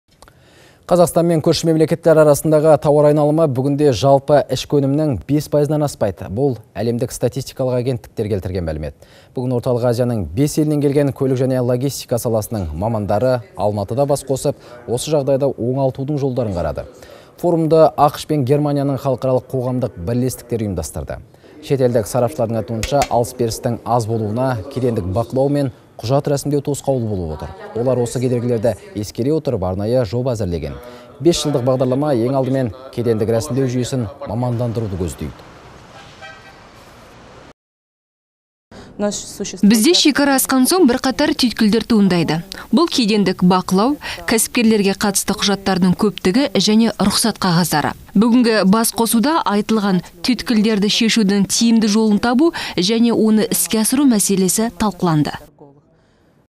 Орталық Азия елдері арасындағы тауар айналымы 15 пайызға дейін артуы мүмкін
Қазақстан мен көрші мемлекеттер арасындағы тауар айналымы бүгінде жалпы ішкі өнімнің 5 пайызынан аспайды. Бұл әлемдік статистикалық агенттіктер келтірген мәлімет.

Бүгін  Орталық Азияның 5 елінен келген көлік және логистика саласының мамандары Алматыда бас қосып, осы жағдайды оңалтудың жолдарын қарады.

Форумды АҚШ пен Германияның халықаралық қоғамдық бірлестіктері ұйымдастырды.

Шетелдік сарапшылардың айтуынша, алыс-берістің аз болуына кедендік бақылау мен құжат рәсімдеу тосқауыл болып отыр. Олар осы кедергілерді ескере отырып, арнайы жоба әзірлеген.

Бес жылдық бағдарлама ең алдымен кедендік рәсімдеу жүйесін мамандандыруды көздейді.

Айжан Бейсенова, USAID агенттігінің Орталық Азиядағы көлік және логистика маманы

-Бізде шекара асқан соң бірқатар түйткілдер туындайды. Бұл кедендік бақылау, кәсіпкерлерге қатысты құжаттардың көптігі және рұқсат қағаздары. Бүгінгі бас қосуда айтылған түйткілдерді шешудің тиімді жолын табу және оны іске асыру мәселесі талқыланды.